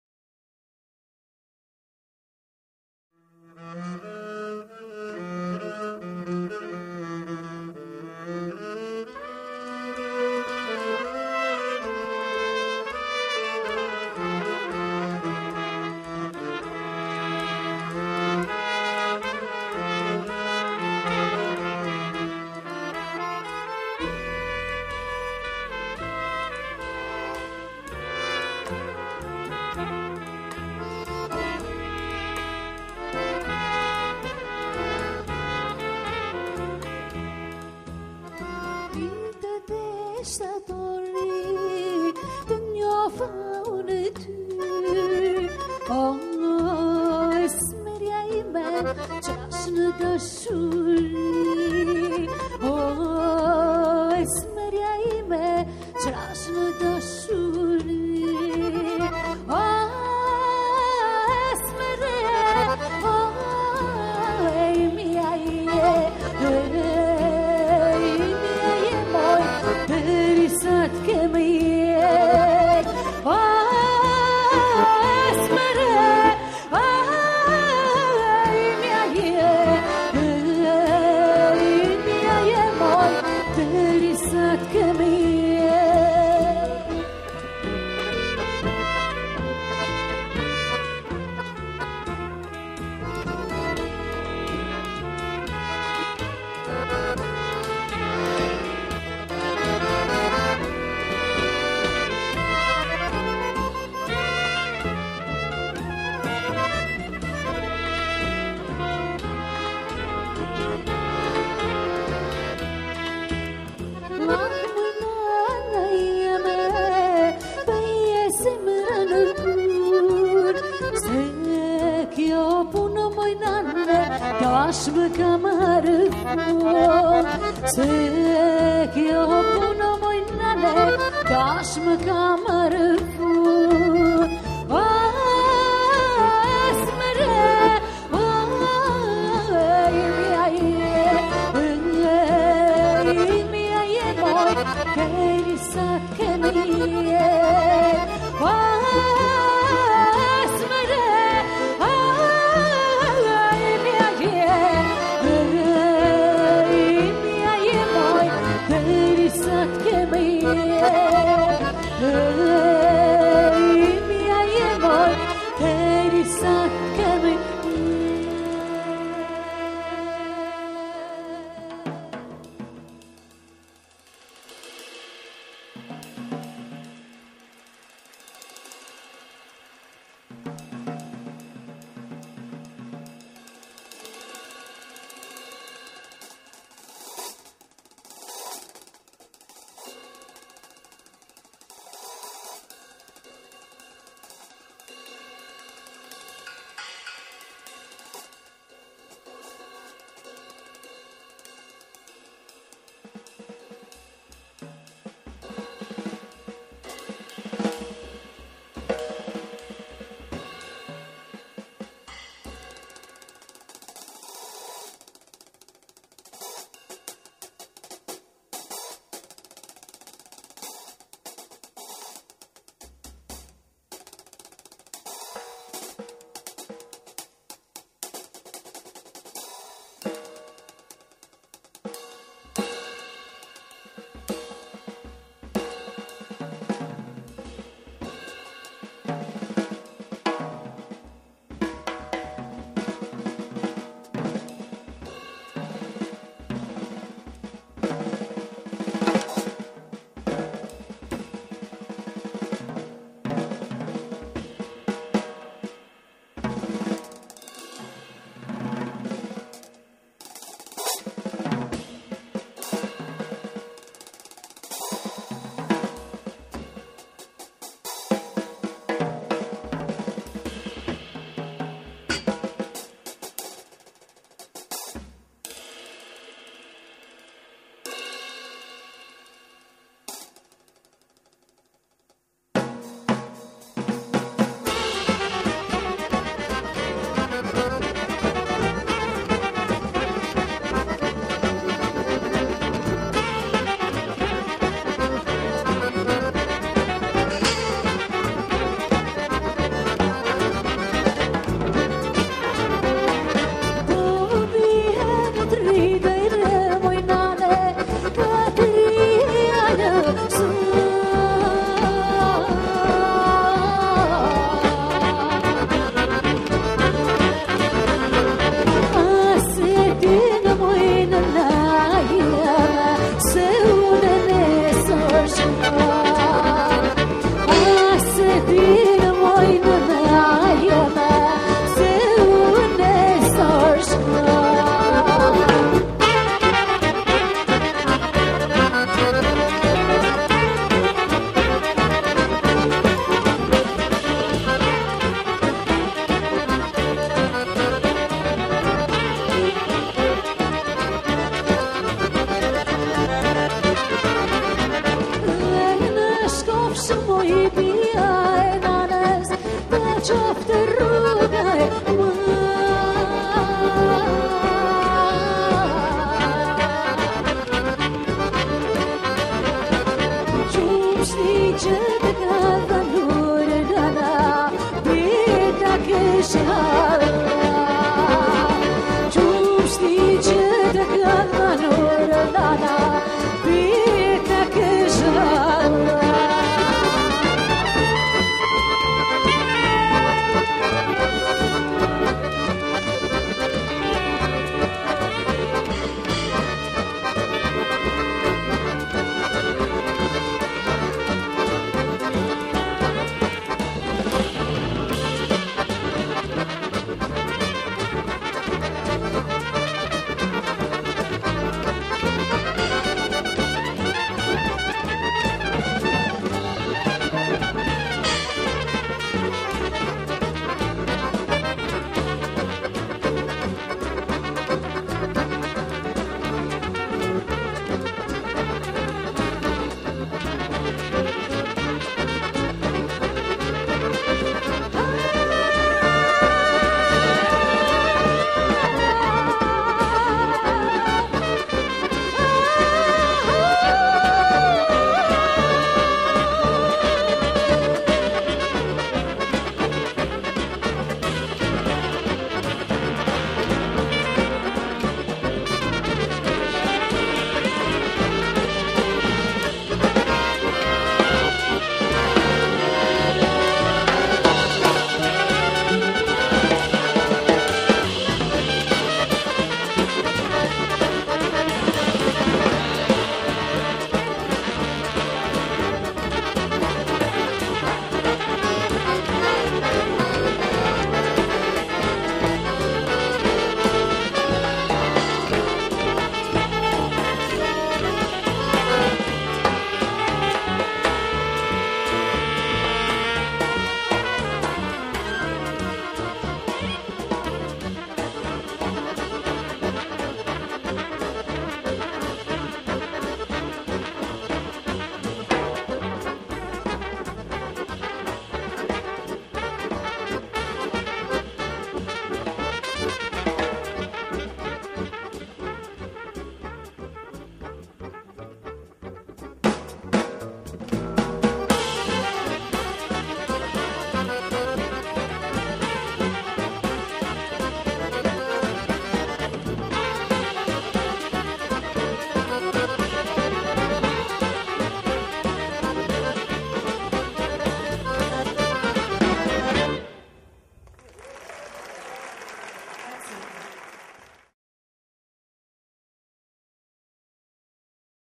Jazz in Veglie - Admir Shkurtaj e i Talea
Ultimo appuntamento di Jazz in Veglie con Admir Shkurtaj e i Talea. Realizzazione audiovisiva a cura di ACMElab